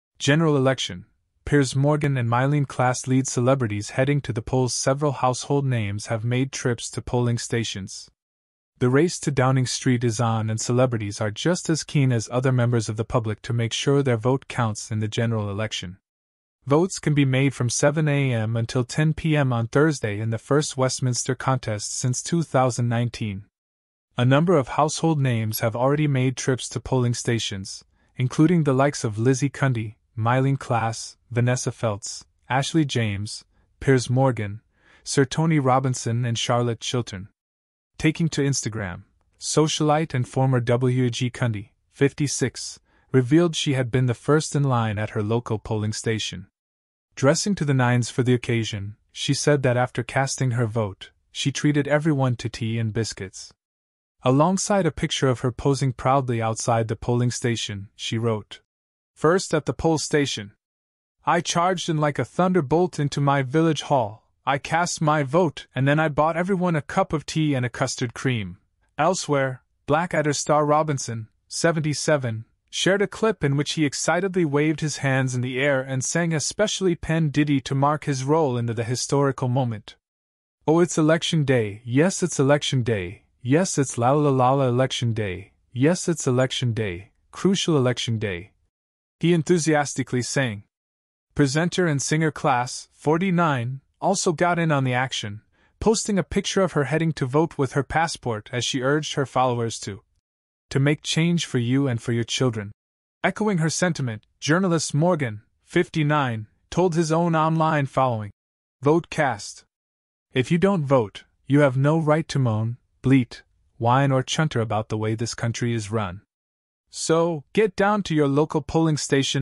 General election: Piers Morgan and Myleene Klass lead celebrities heading to the polls
CELEBRITIES INCLUDING MYLEENE KLASS, PIERS MORGAN AND LIZZIE CUNDY HAVE VOTED IN THE 2024 GENERAL ELECTION